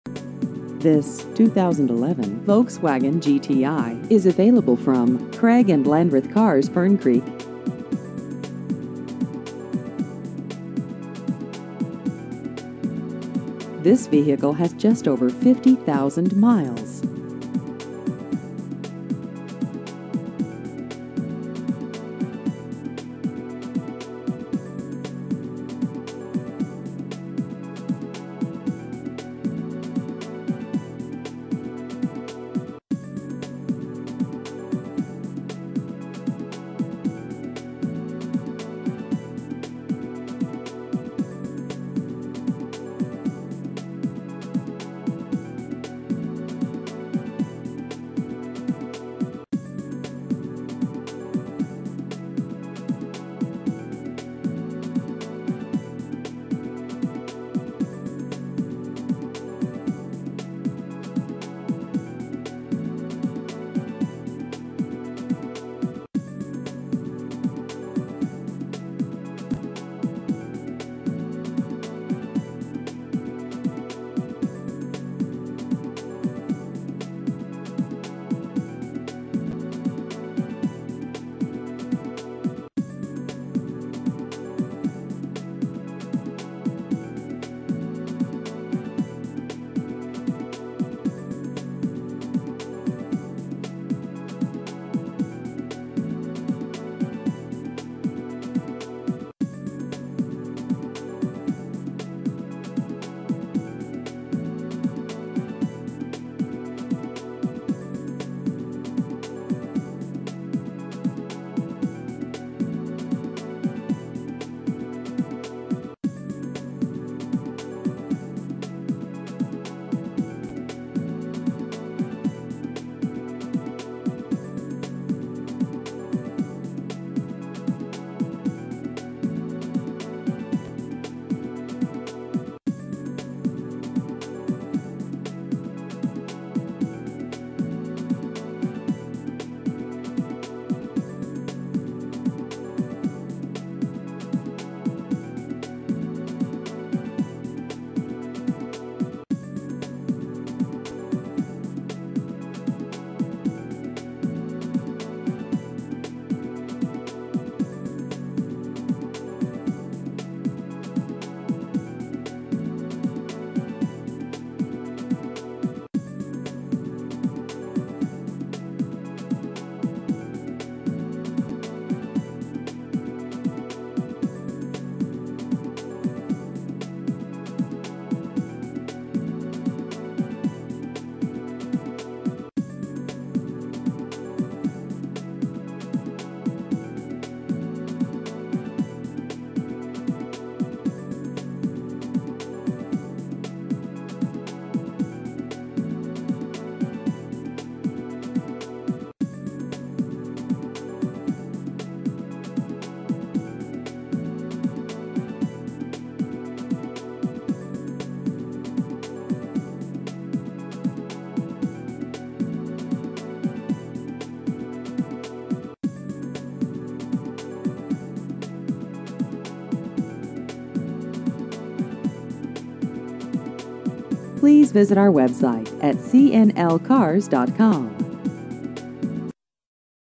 2011 Volkswagen GTI Used Cars Louisville KY
This 2011 Volkswagen GTI is available from Craig and Landreth Cars - Fern Creek.